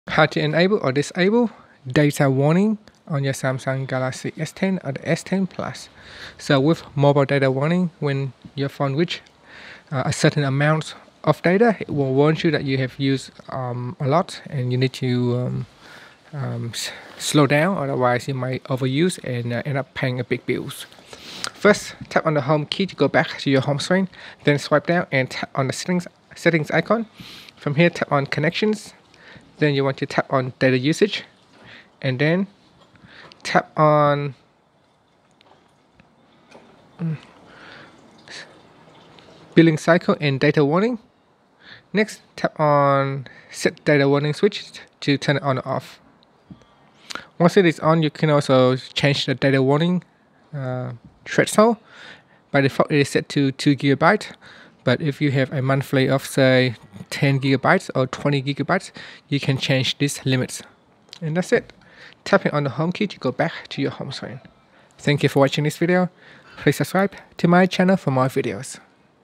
Samsung Galalaxy S10 / S10+: How to Enable / Disable Mobile Data Warning
Learn how you can enable or disable Mobile Data Warning on Samsung Galaxy S10 / S10e / S10+.